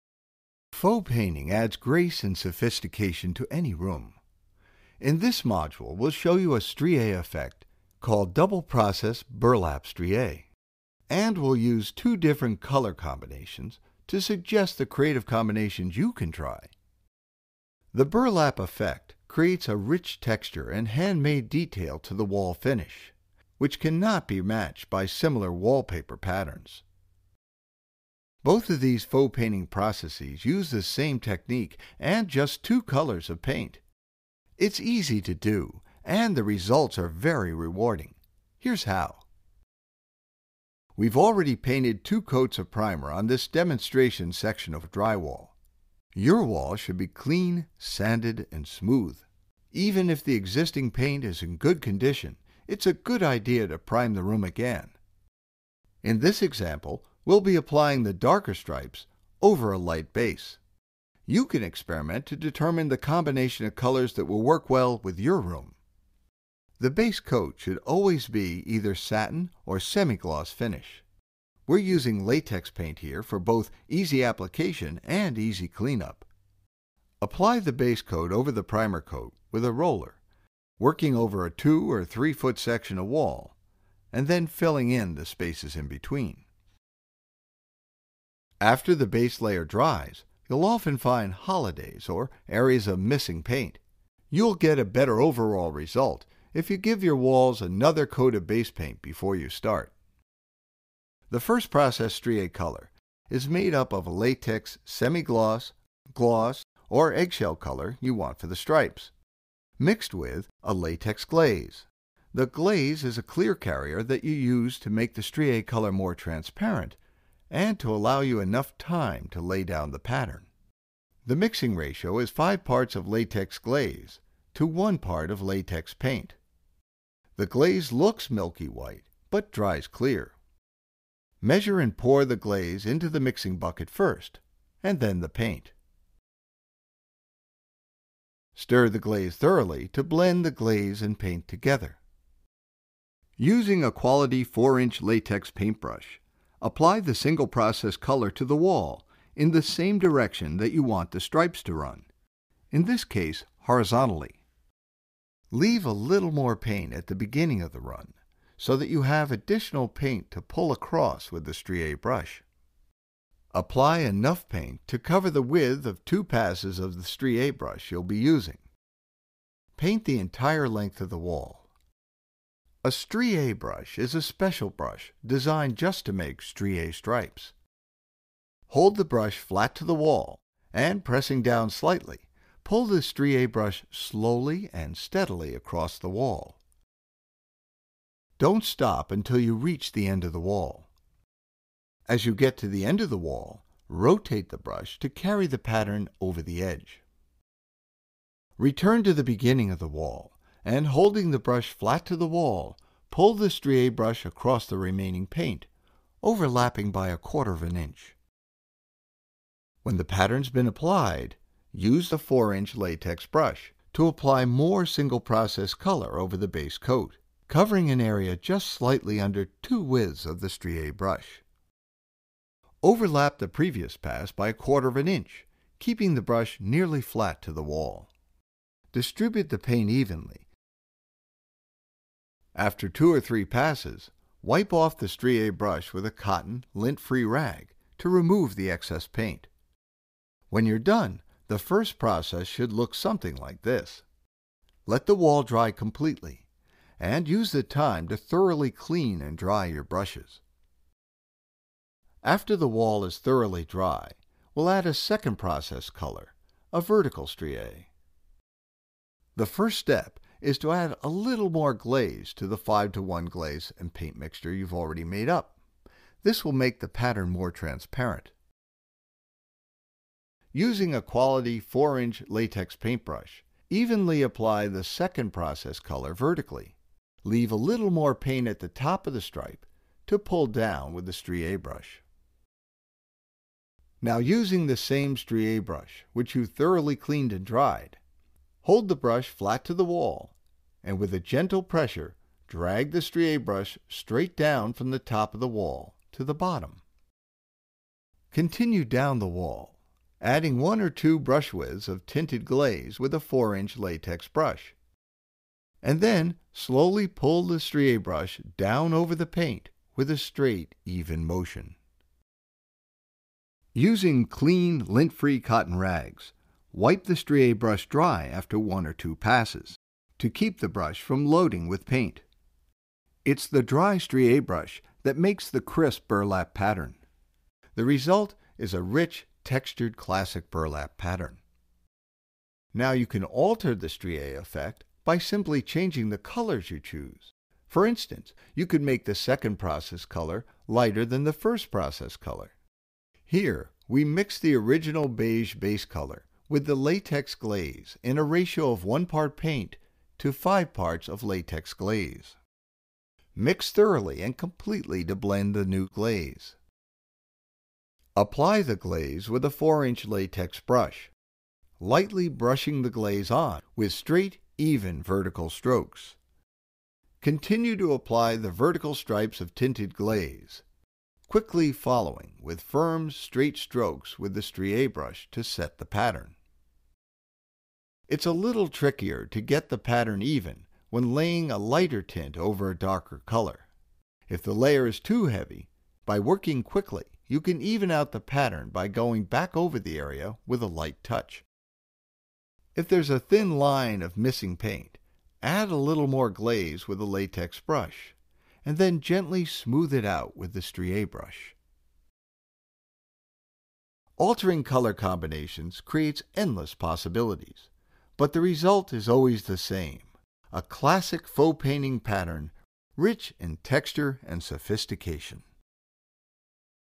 How to Apply a Double-Process Strie Faux Painting Technique
Faux painting techniques, like this double-process burlap strie, can add grace and sophistication to any room.